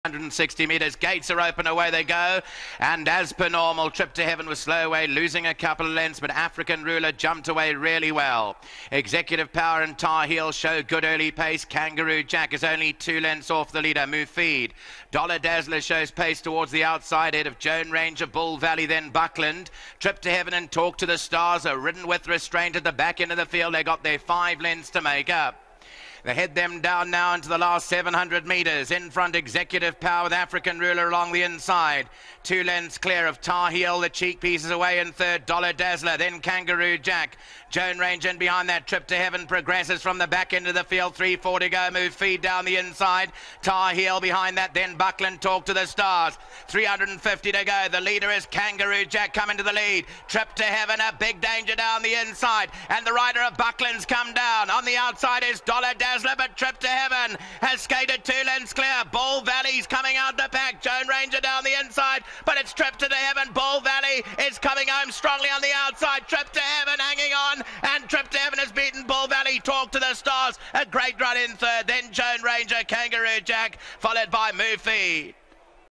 2016.11.26 The Citizen Merchants (RSA) - Trip to Heaven
4yo&up, Turf 1160 Meters Gr.2
1.Trip to Heaven - S'Manga Khumalo - S. G. Tarry 1:07.78
2.Bull Valley
3.Talktothestars
4.Joan Ranger
5.Kangaroo Jack 8.African Ruler 10.Tar Heel